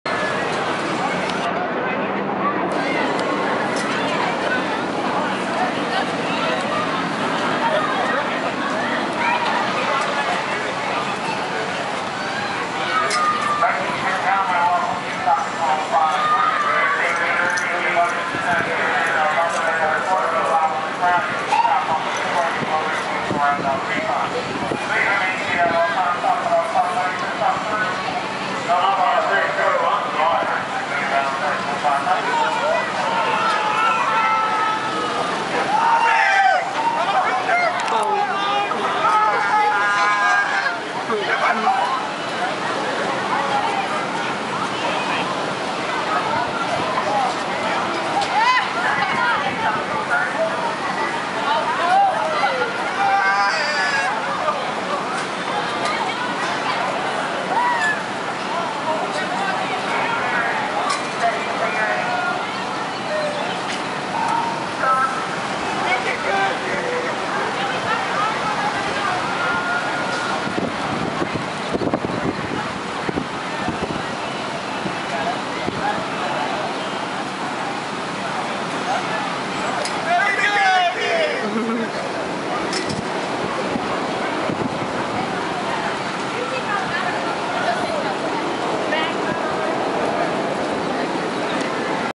Dive Devil FALL @ Six Flags CALIFORNIA..
CRAZIEST DROP EVER...

took a quick trip to Six Flags Magic Mountain in Valencia California..